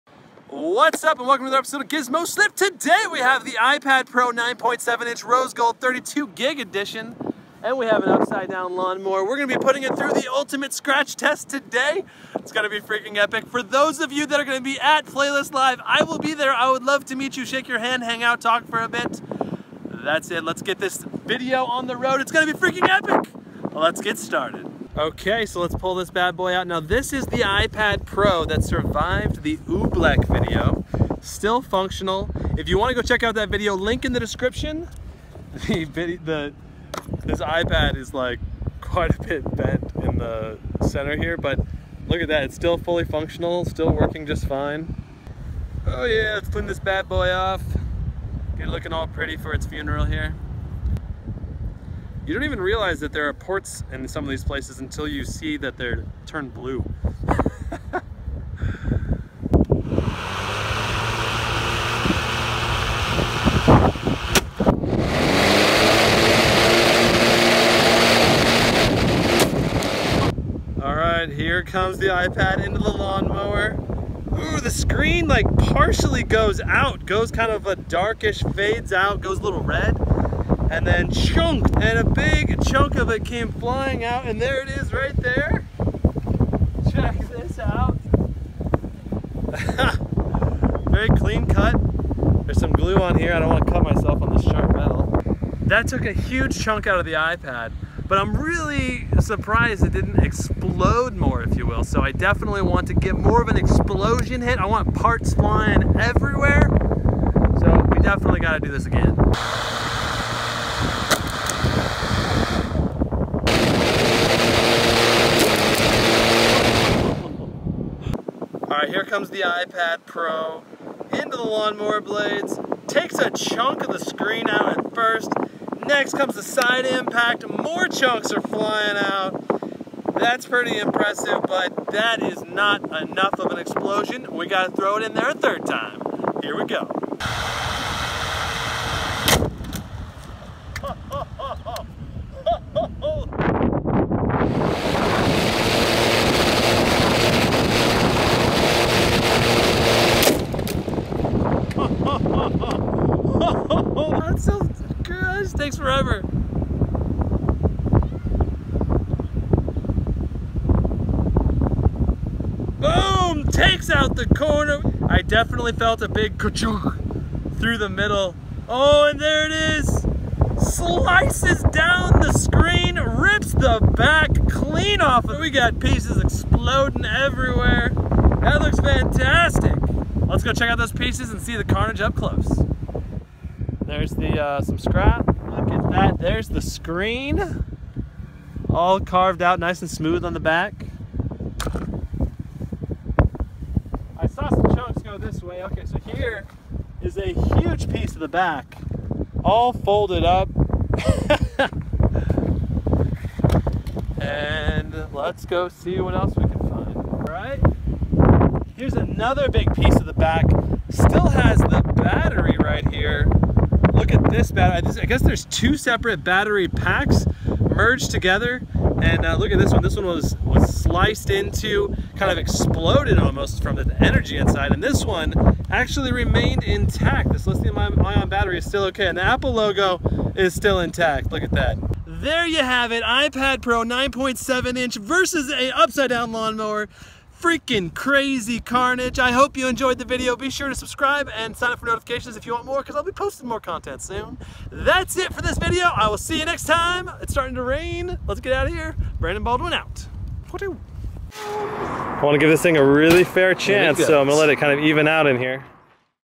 iPad Pro 9.7" Upside Down Lawn Mower Scratch Test! - GizmoSlip
The iPad Pro 9.7" was put to an excruciating scratch test this week against an upside down lawn mower! The pain train came to this iPad Pro even though it survived the crazy Oobleck test last week. What crazy scratch, drop, or durability test should come next? Post your opinion down below!

Follow me on other social media for previews before videos are uploaded!